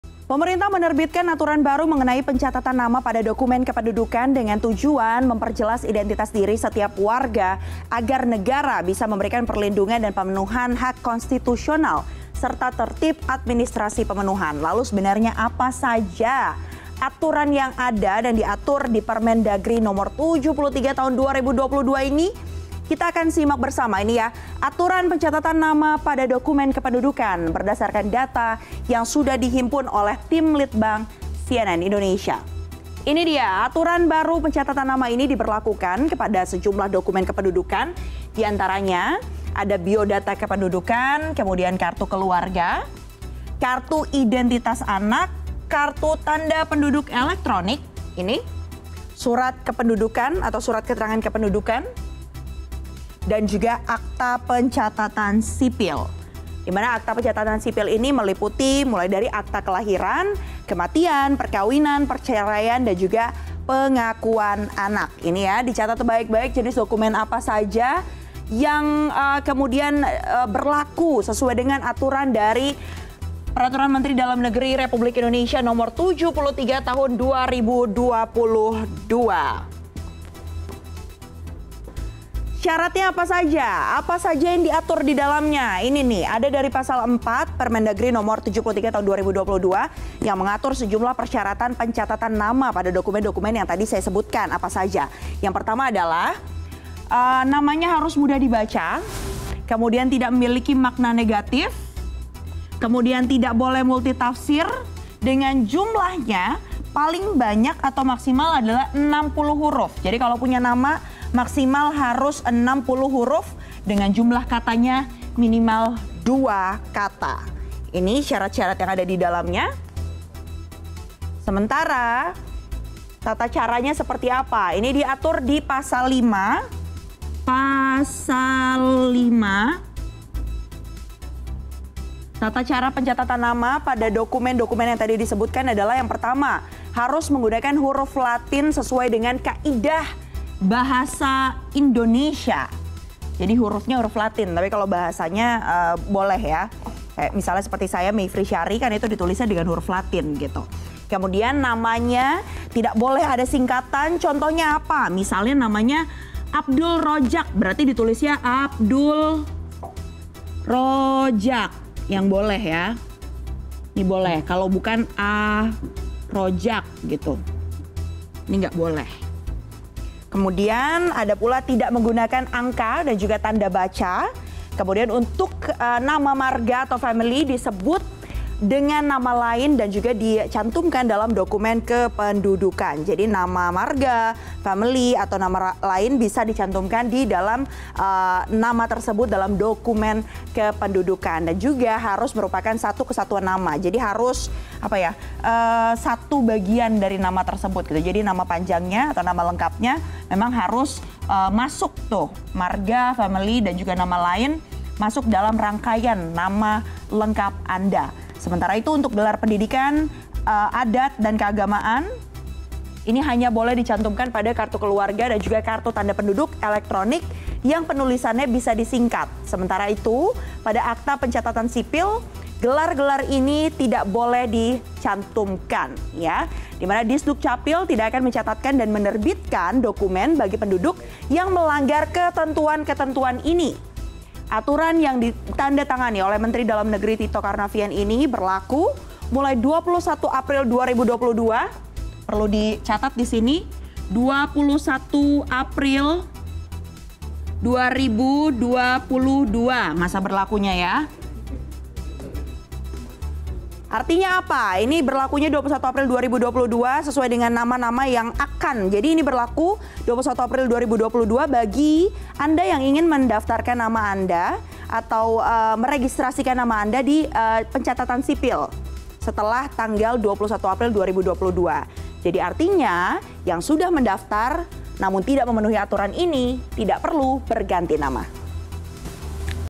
Ini Nih Aturan Pencatatan Nama Pada Dokumen Kependudukan
Pemerintah menerbitkan aturan baru mengenai pencatatan nama pada dokumen kependudukan, dengan tujuan memperjelas identitas diri setiap warga agar negara dapat memberikan perlindungan dan pemenuhan hak konstitusional dan tertib administrasi pemenuhan.

Apa saja aturan yang ada di permendagri nomor 73 tahun 2022?. Berikut data yang dihimpun tim litbang Cnn Indonesia.